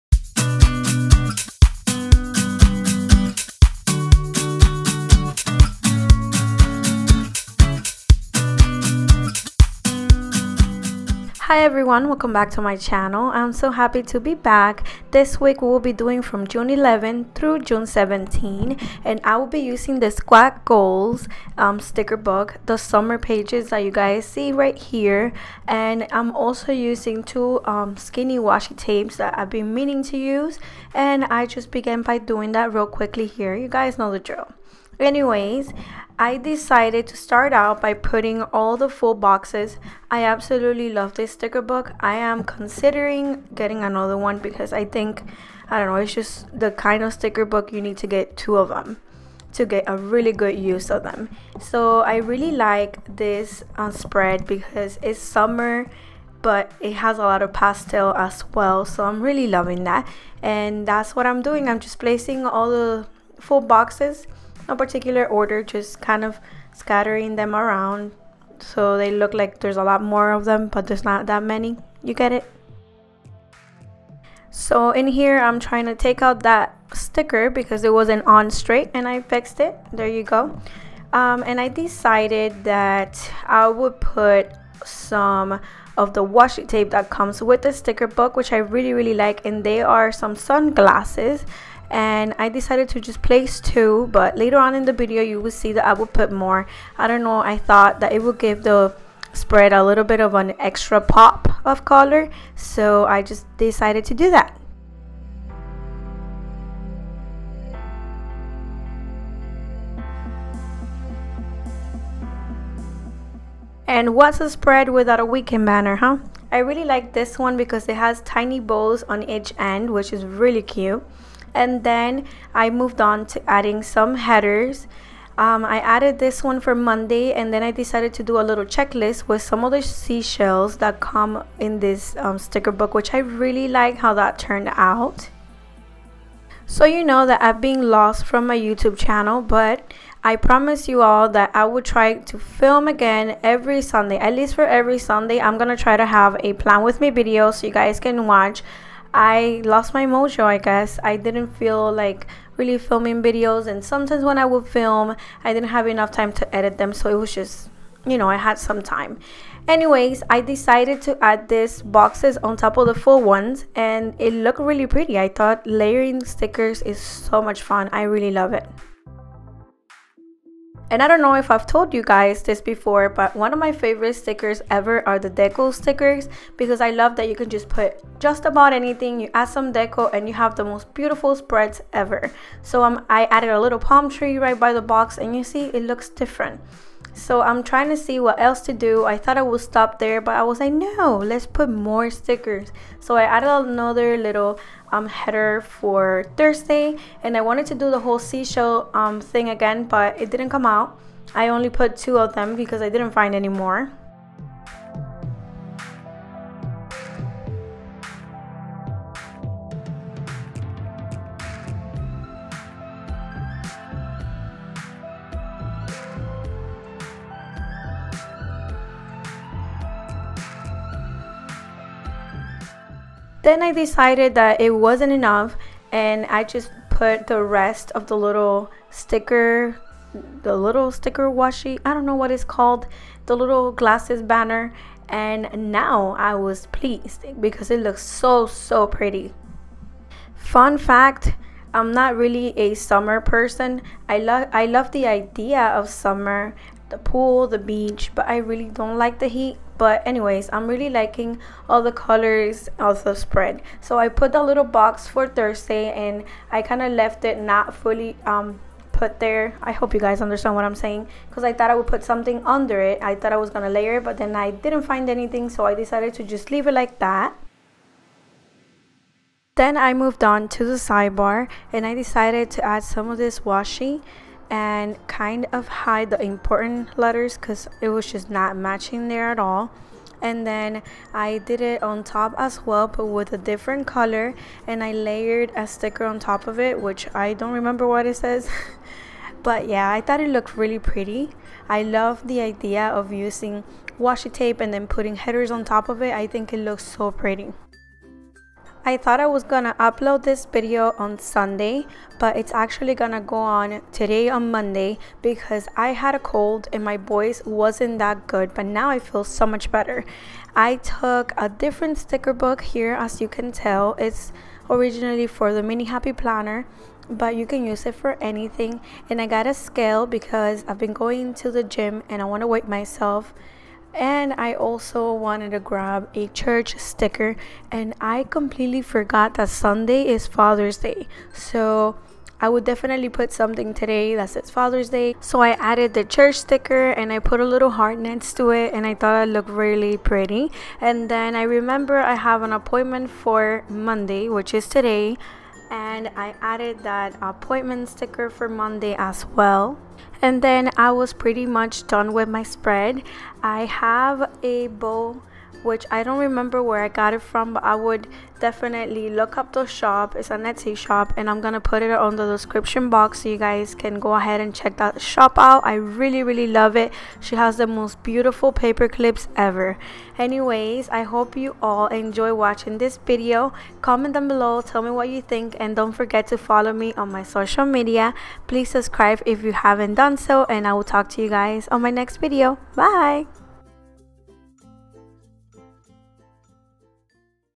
Plan with me| The Happy Planner (No Etsy Kit) Squad Goals!
Hi planner friends!
I'm back for a plan with me, I hope you enjoy watching this one and get some planner inspiration. God Bless
xoxo
Lesly

"If you seek me early and diligently, you will find ME"
-Proverbs 8:17

Bow Paper Clip

Sticker Book

Squat Goals (The Happy Planner)